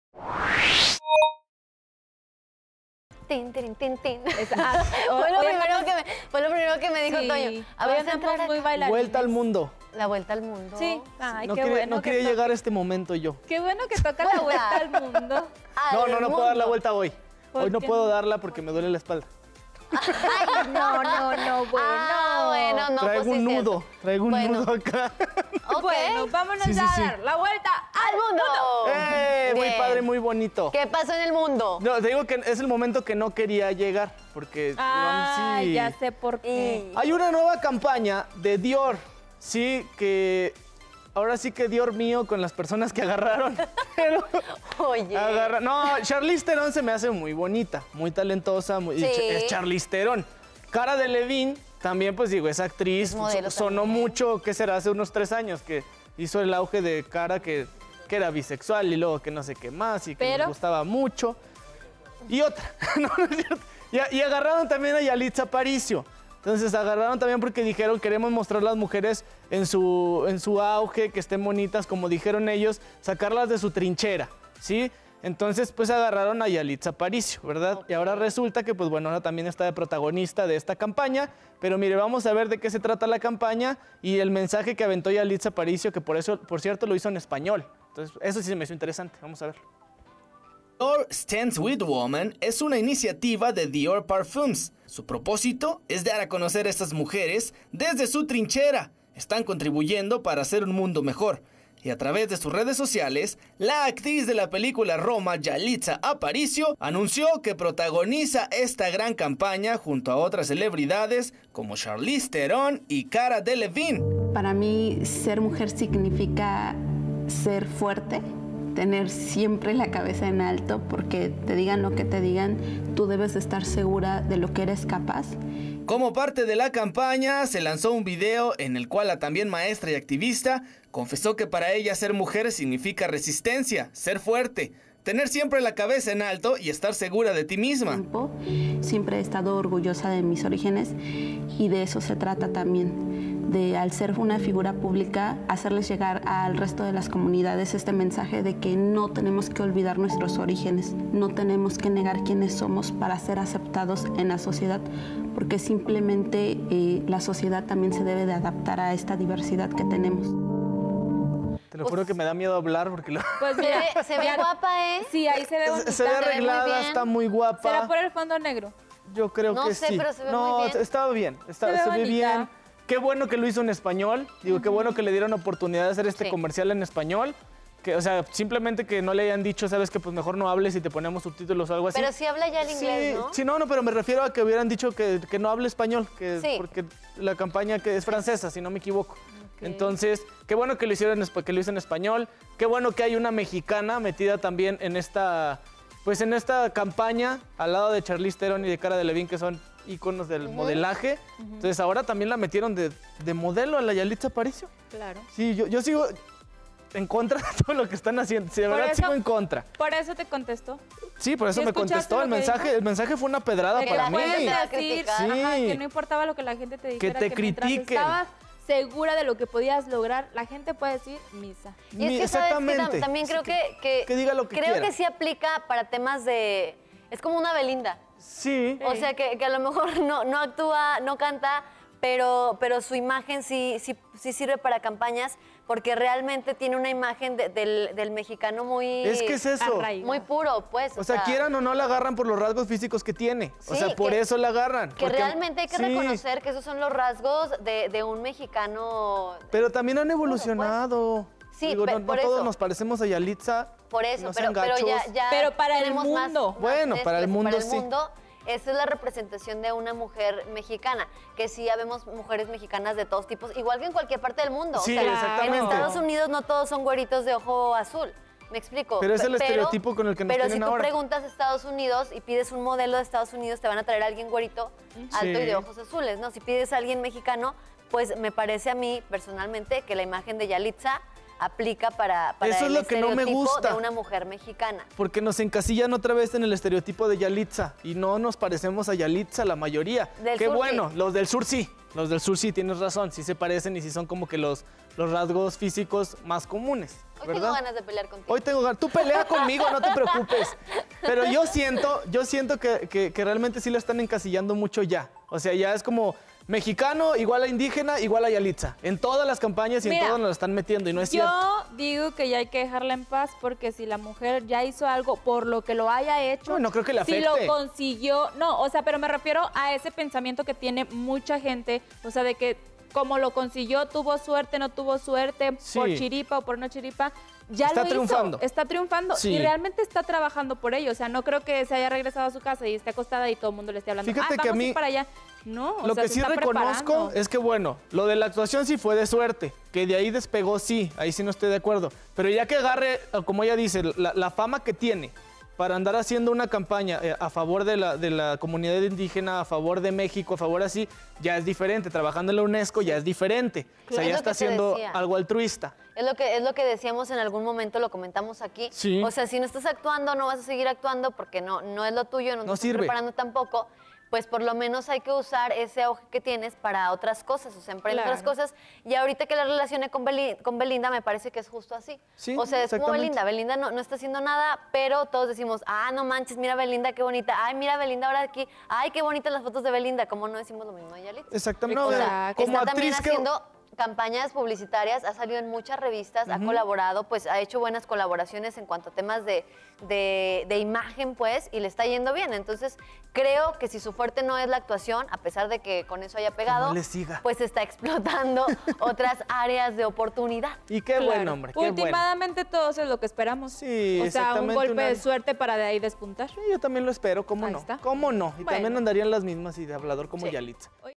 Yalitza Aparicio en campaña de Dior
YALITZA ESTARÁ EN UNA IMPORTANTE CAMAPAÑA

La actriz mexicana Yalitza Aparicio estará dentro de la campaña de Dior junto a otras grandes exponentes del cine y la moda. No cabe duda de que este es un gran logro para la oaxaqueña y en RCG Media estamos orgullosos de ella por esta increíble oportunidad... bueno, Fede no, pero no nos importa, nosotros sí que lo estamos.